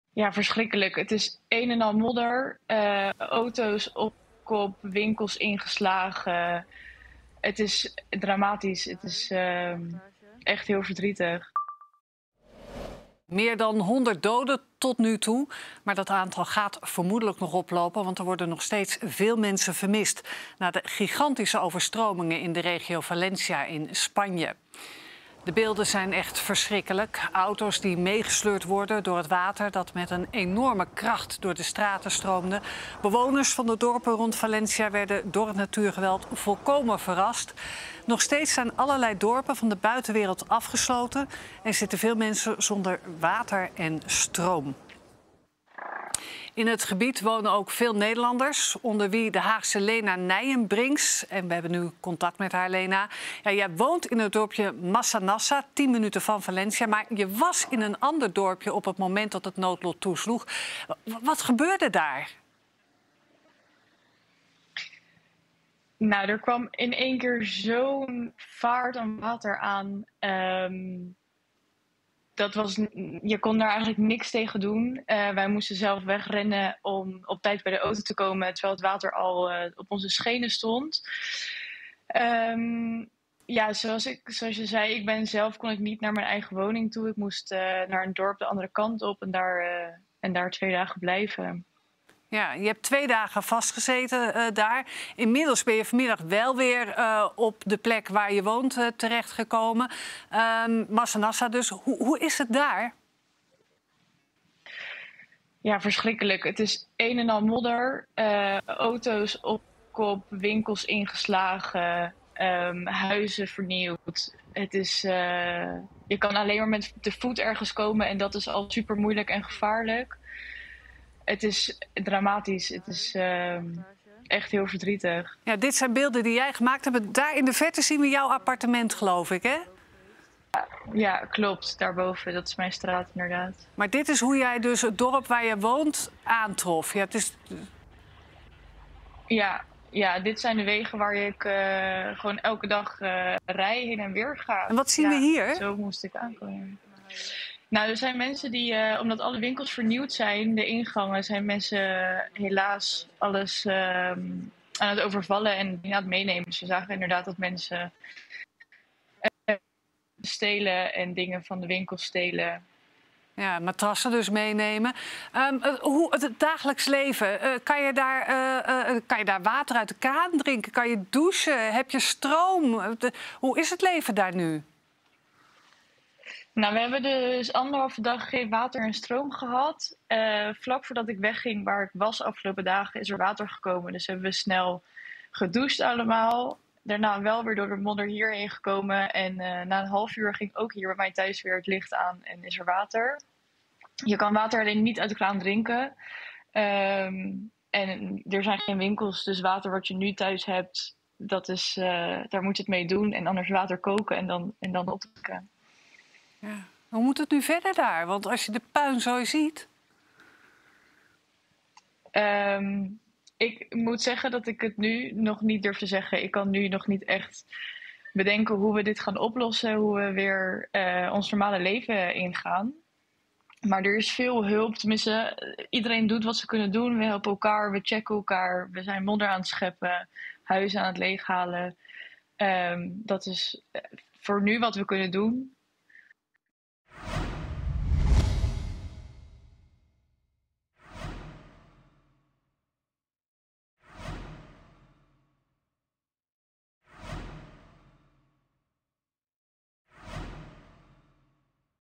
Lena (23) woont in Spanje en zat twee dagen vast door overstromingen
Extreme regenval leidde begin deze week tot watersnood in Spanje. Het dodental is inmiddels gestegen naar 205. Ook de woonplaats van Lena uit Den Haag - op tien minuten rijden van Valencia - is getroffen door ongekend natuurgeweld.

Eerder dreven er auto's door haar straat. En nu het water deels is gezakt, slaan mensen aan het plunderen.